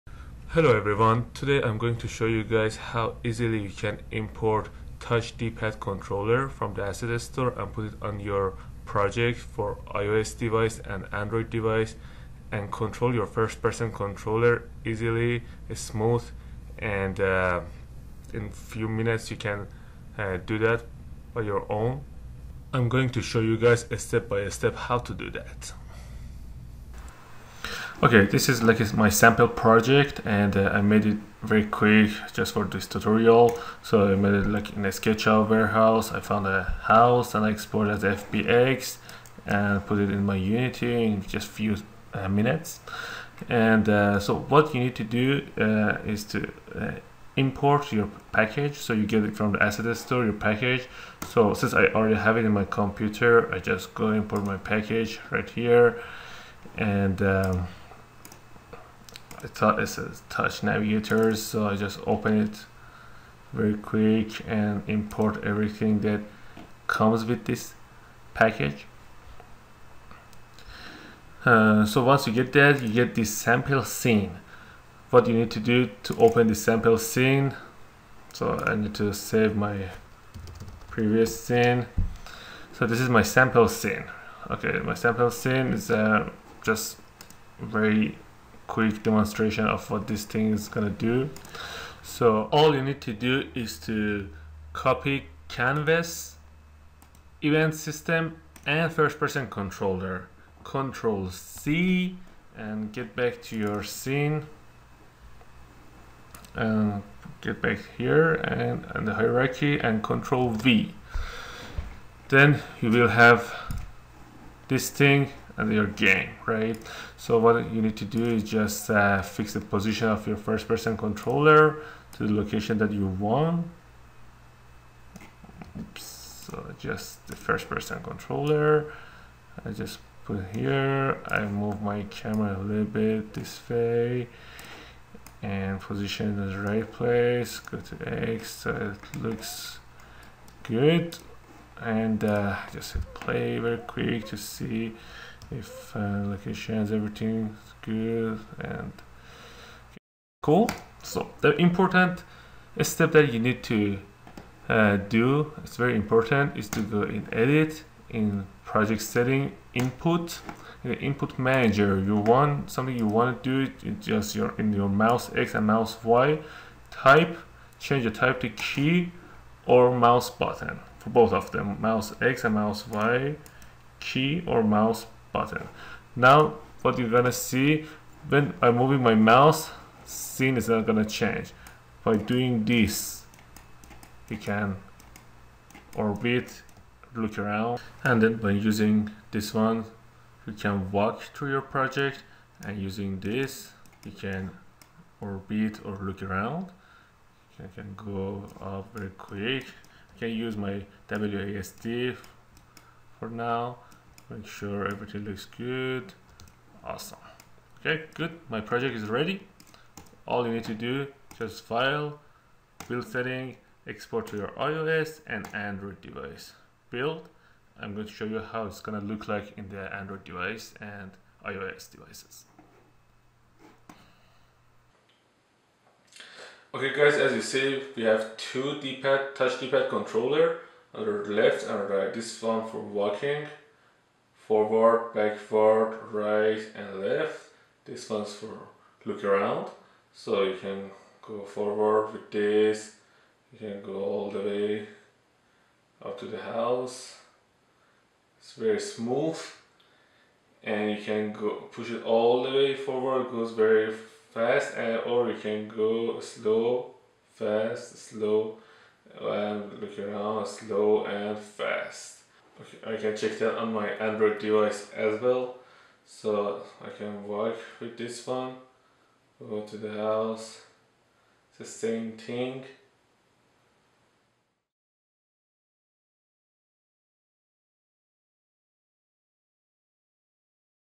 Touch Dual DPAD Controller - Create Android & iOS walkthrough for Architecture/3D model
You can easily import this package into your game/3D model using Unity3D game engine software and have ability to control/navigate your First Person controller with iOS(iPad/iPhone) and any Android devices.
It gives you ability to touch left D-pad for moving forward, backward, left and right and also touch right D-pad for look around 360 -180.
Movement is very smooth and depends on how hard you push it, goes fast and slow.